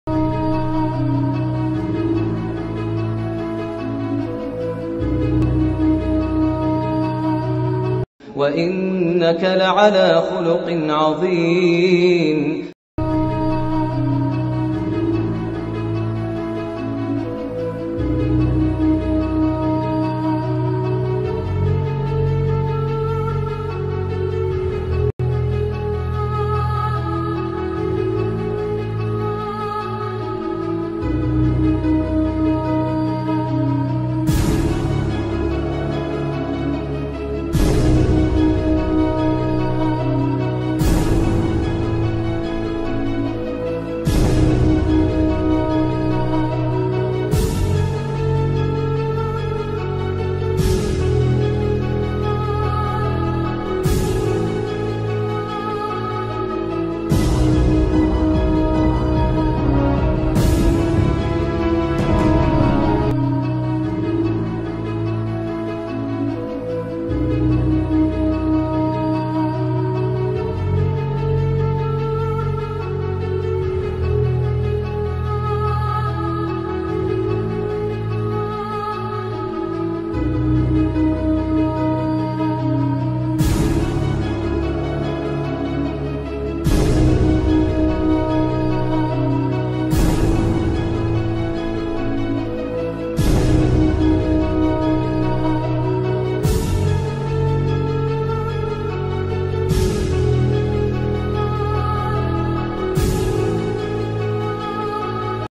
في ذكرى المولد_النبوي ماذا قال علماء الغرب عن رسول الله؟!
شهادات عجيبة لعلماء غربيين غير مسلمين في حق النبي -صلى الله عليه وسلم-، وصد من قال: الحق ما شهدت به الخصوم..